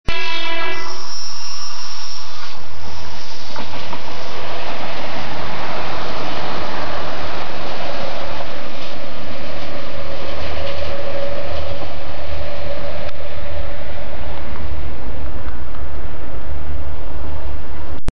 a train
a train going into a tunnel